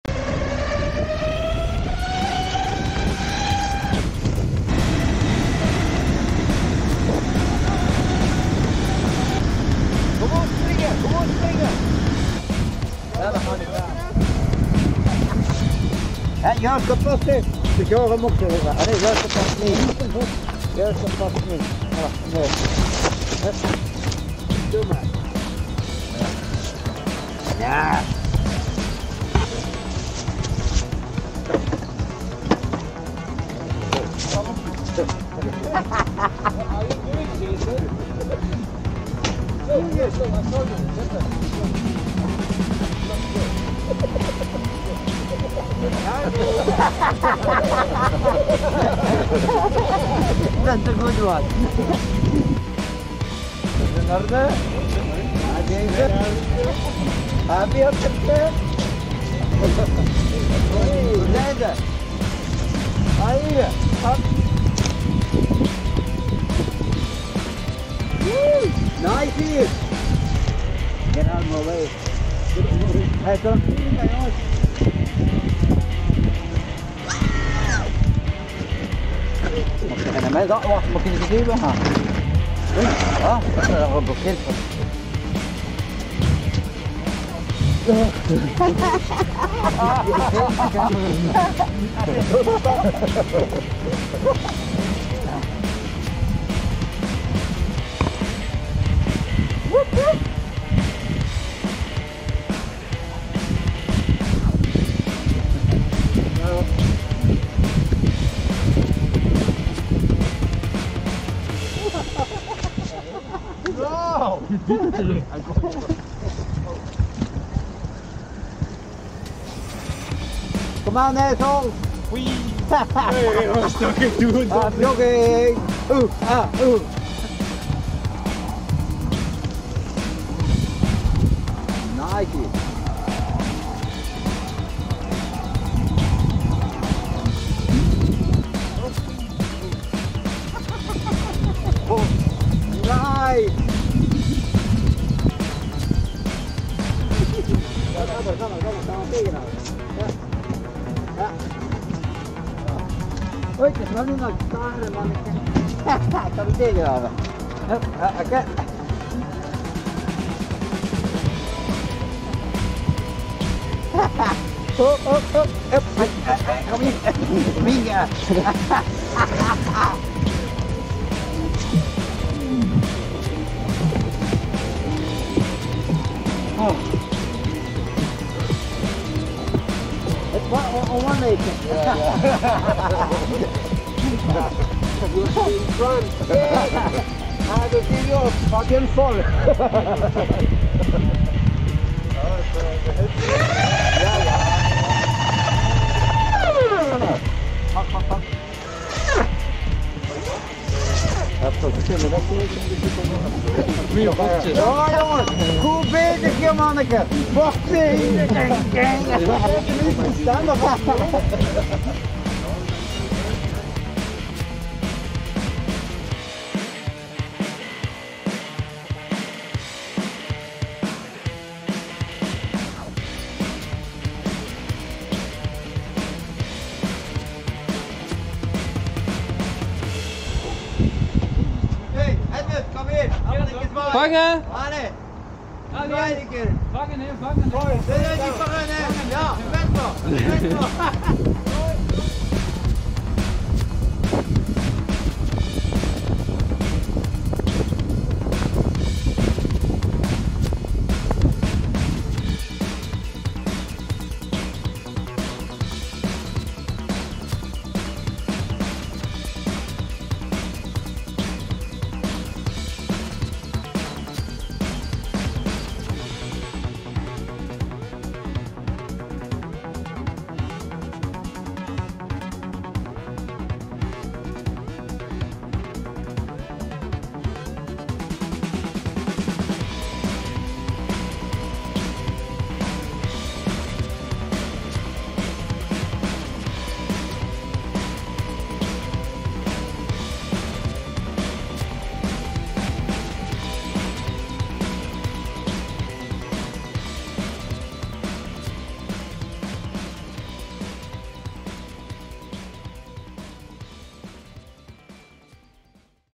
Belgium Euc Riders and Esk8.eu group ride in Ghent
We had a fun day and evening in and around the Ghent city area. Belgium Euc Riders and Esk8.eu